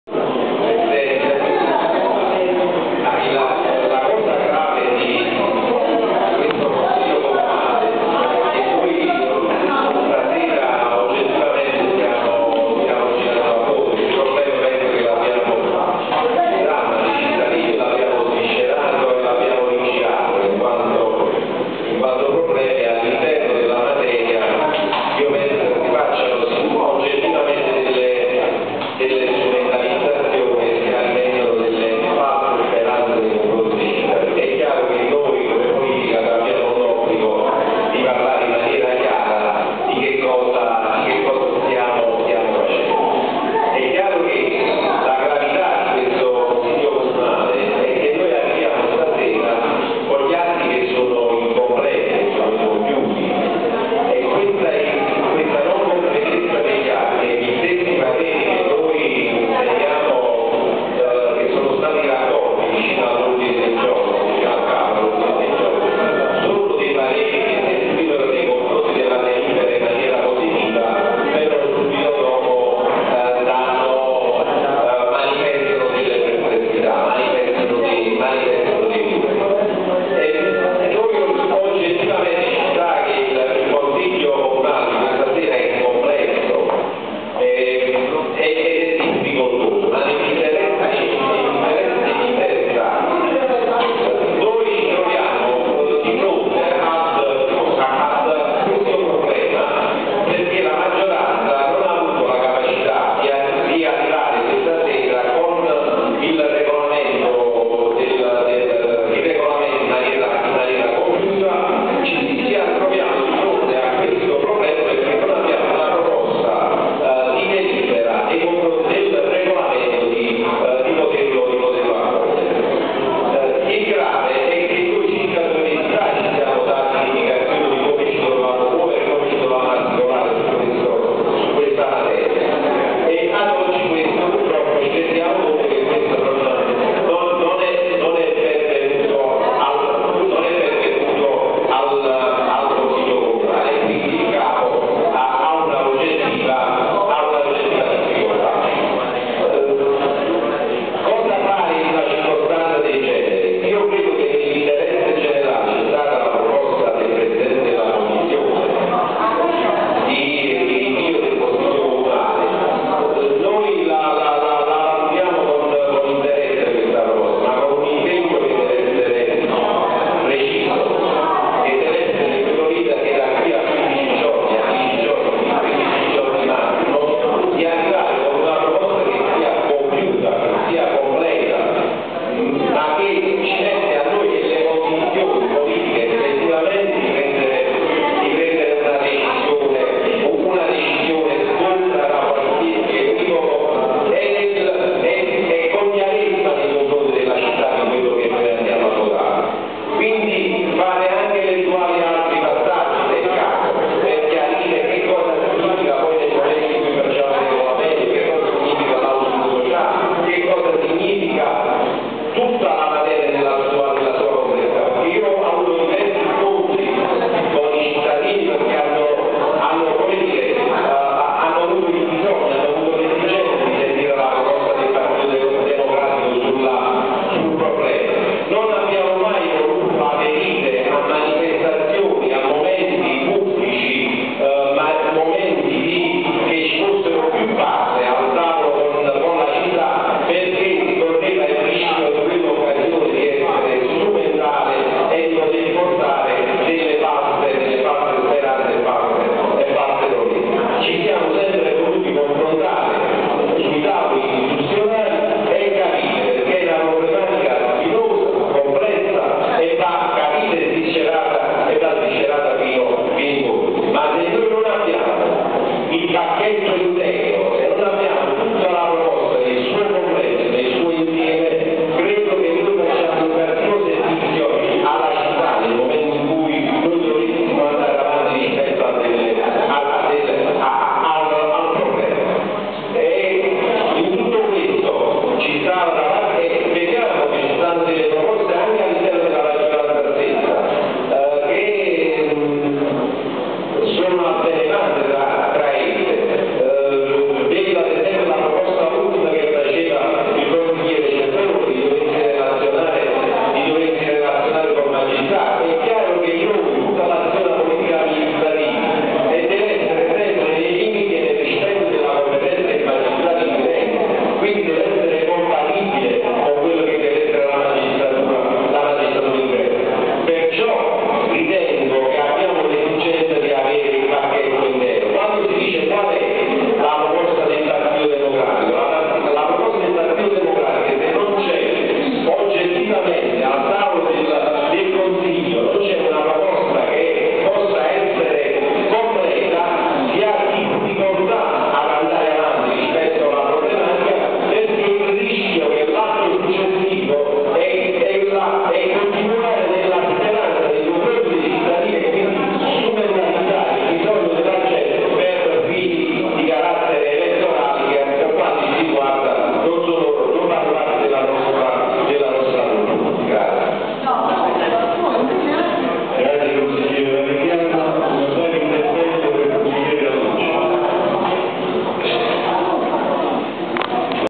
Casoria 12/09/2014 - Consiglio Comunale (17)
"Casoria Ieri & Oggi online" presenta:
Casoria 12 settembre 2014, il consiglio comunale è convocato in sessione straordinaria di prima convocazione per il giorno 12  settembte 2014 alle ore 15,00 presso la sala Consiiiare della  Sede Comunale sita in P.za Cirillo per la trattazione dei seguenti argomenti:
1) Recepimento art.1, comma 65, L.R. Campania n.512013.'atto
    d'indinzzo;
2) Approvazione Regolamento recepimento L'R' Campania
    n°  5/ 12013  ;
3) Elezione del vice Presidente del Consiglio Comunale.
Intervento del consigliere Giovanni Marigliano (Pd)